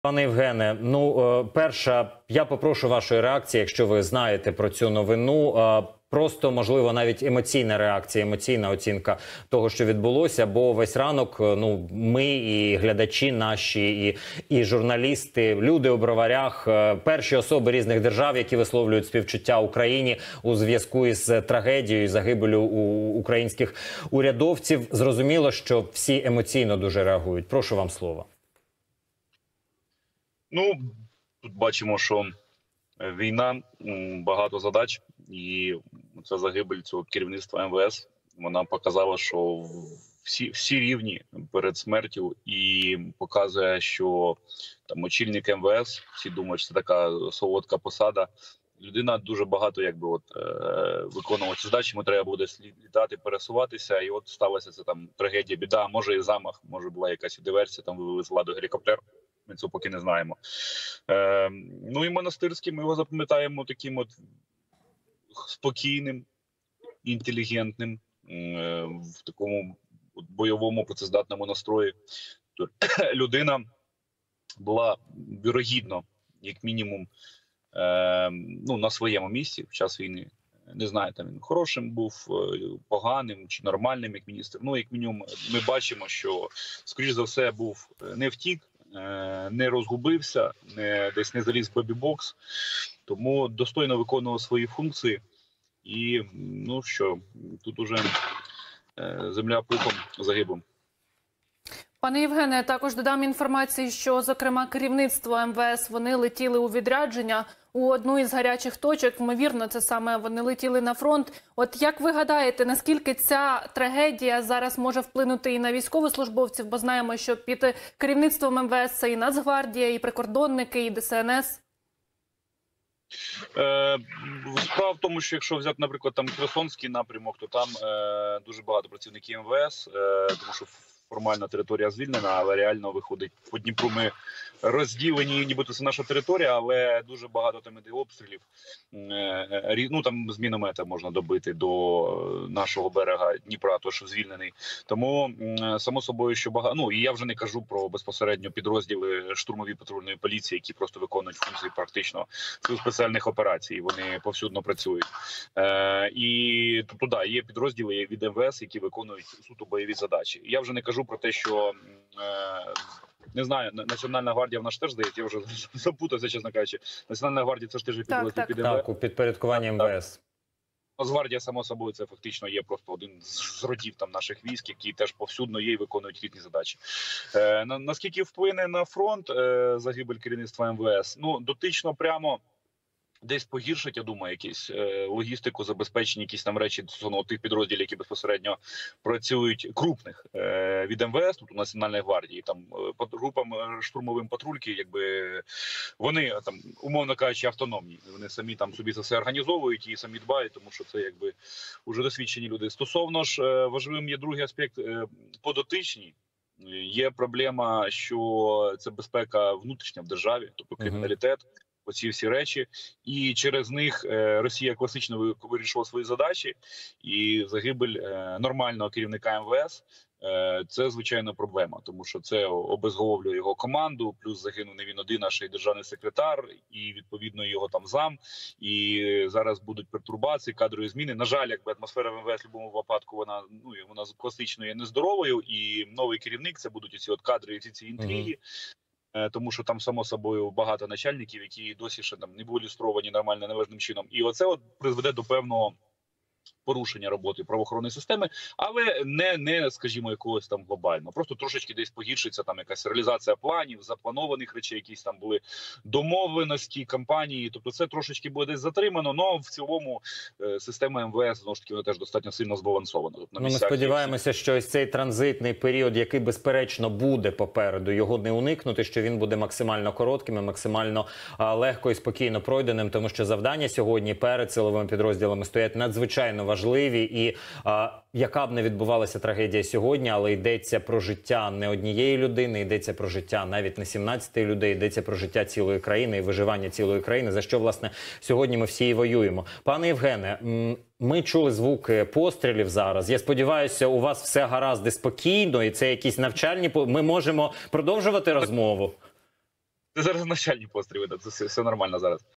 Як загибель Монастирського вплине на фронт та внутрішню безпеку / Карась
В ефірі: Євген Карась, військовослужбовець ЗСУ